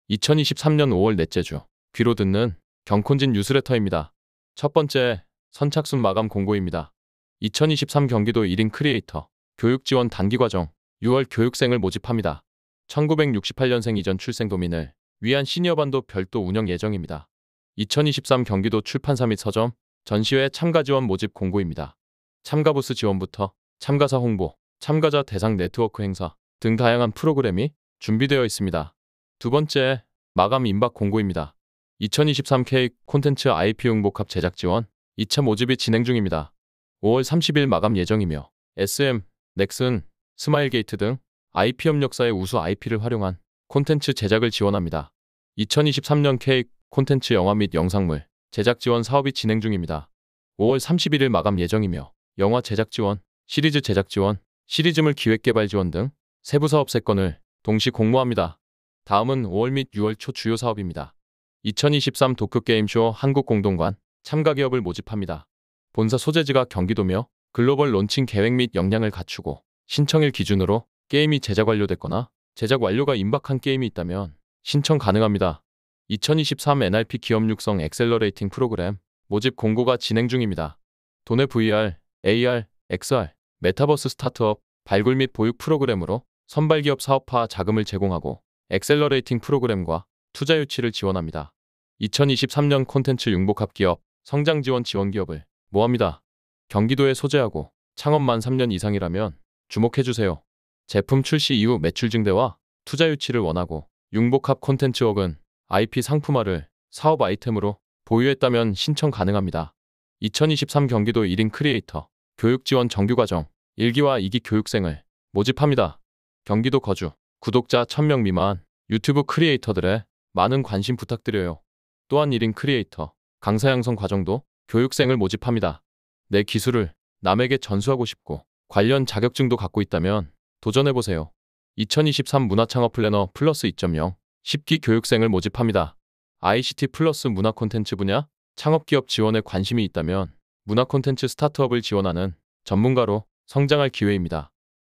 🎮 도쿄게임쇼 출품 가능? ｜경기콘텐츠진흥원 5월 넷째주 지원사업 안내
💌경콘진 5월 넷째 주 뉴스레터💌

1️⃣선착순 마감!
00:07 2023 경기도 1인 크리에이터 단기과정

2023 경기도 출판사·서점 전시회 참가지원 (언리미티드에디션15)모집 공고

2️⃣ 마감 임박 사업
00:31 2023 K-콘텐츠 IP융복합 제작지원 2차 모집

2023년 K-콘텐츠 영화 · 영상물 제작지원

3️⃣ 5월 및 6월 초 주요 사업
01:01 2023 도쿄게임쇼 한국공동관 (경기지역) 참가기업 모집 (~6.2)

2023 NRP 기업육성 엑셀러레이팅 프로그램 모집 공고 (~6.4)

2023년 콘텐츠 융복합 기업 성장지원 모집 (~6.7)

2023 경기도 1인 크리에이터 교육지원 (정규과정) 1,2기 교육생 모집 (~6.12)

2023 경기도 1인 크리에이터 교육지원 (강사양성 과정) 교육생 모집 (~6.12)

2023 문화창업플래너+2.0 10기 교육생 모집 (~6.16)

00:00 ~ 00:07 INTRO
00:08~  00:30 선착순 마감 사업
00:30 ~ 00:59 마감임박사업
01:01 ~ 02:25 5월 및 6월 주요사업